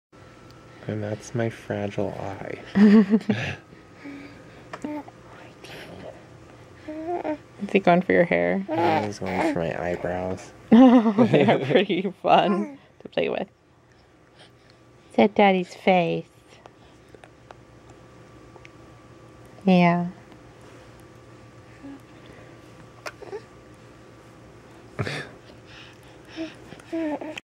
Grabbing Daddy's Face
Fifteen-week-old Damien is just learning to grab - Daddy's face looks like a good target! Watch out for the eyes...